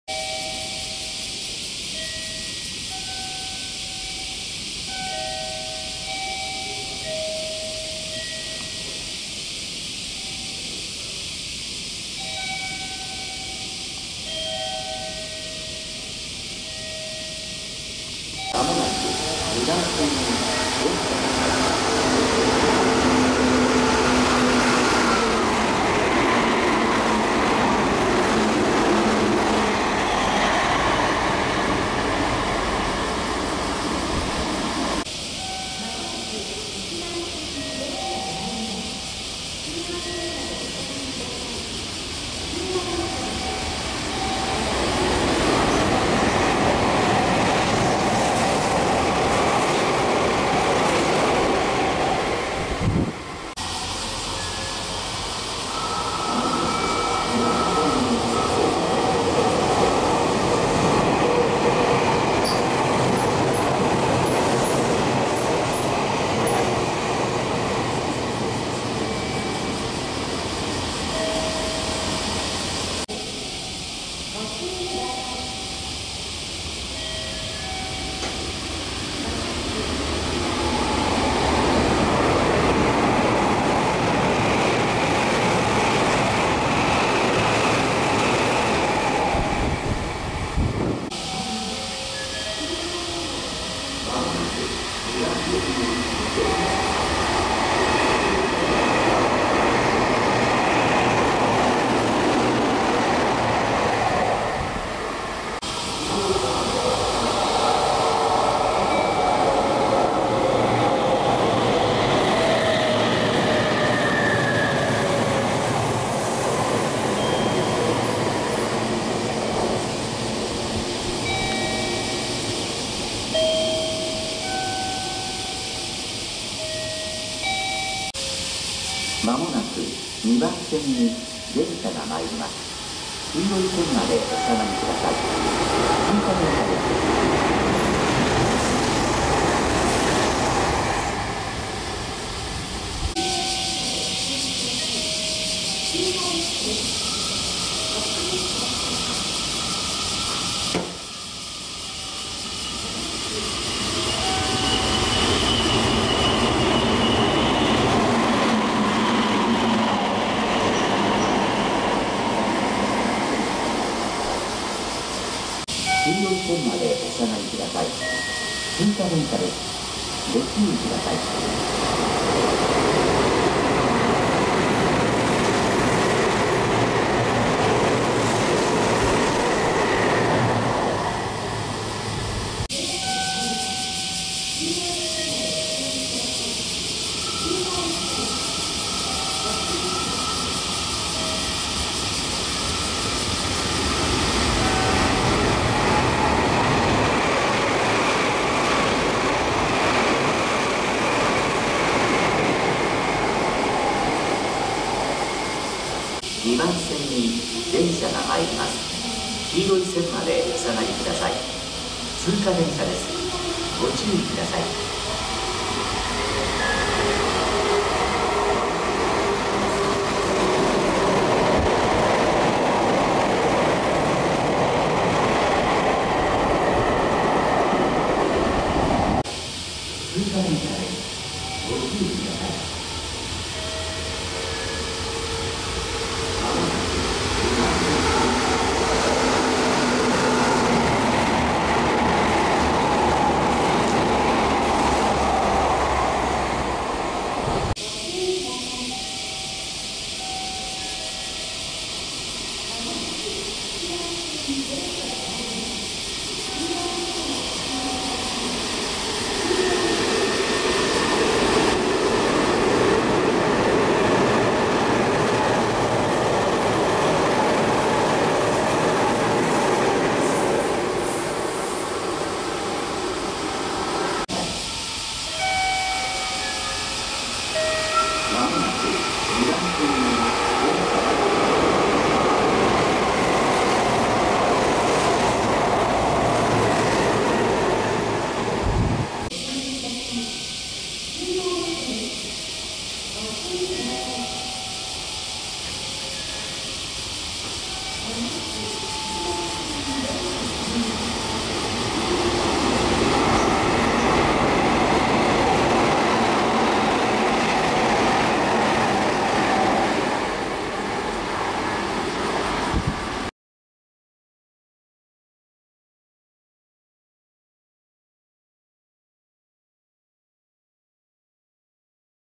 【名鉄常滑線】大同町駅を通過する電車を撮影してきた / 急行・快速急行・特急・ミュースカイ・回送
今回は名鉄常滑線の大同町駅に来ております。
今回の動画では、急行、快速急行、特急、ミュースカイ、回送が通過します。

思いっきり夏ですみませんwww